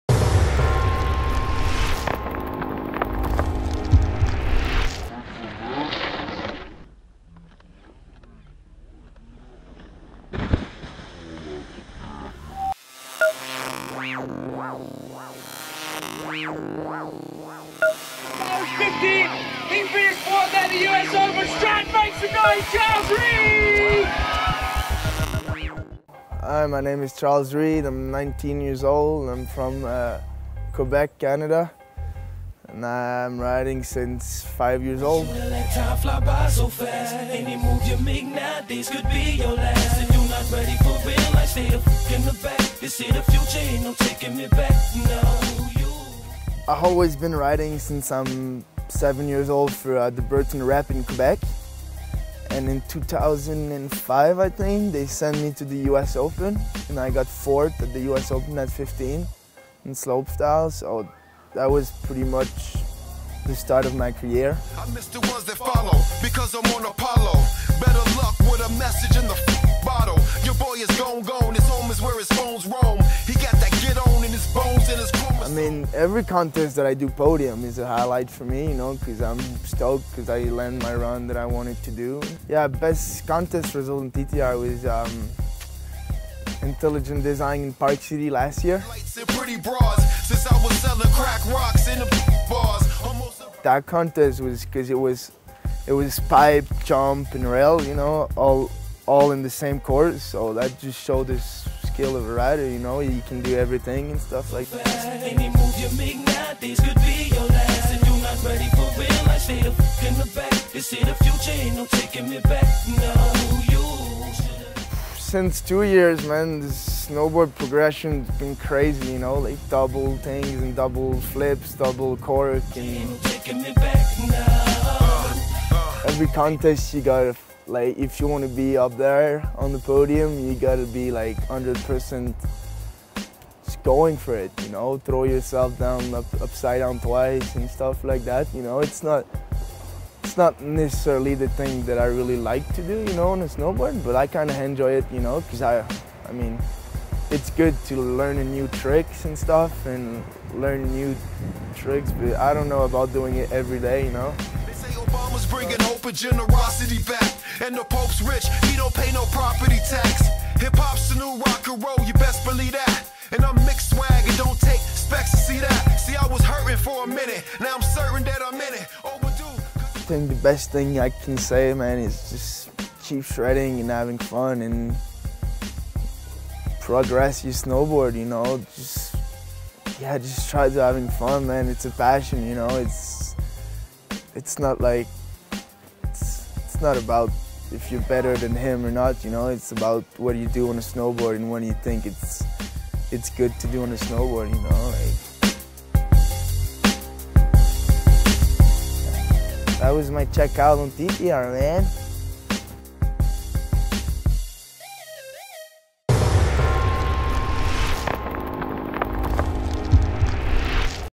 Charles Reid - TTR Rider Profile
Check out Charles Reid's 2009/2010 Swatch TTR World Snowboard Tour Profile. Watch some of the Canadian sensations his best tricks as he talks about the high's and low's of being a professional snowboarder, the progression of modern snowboarding and the Swatch TTR World Snowboard Tour.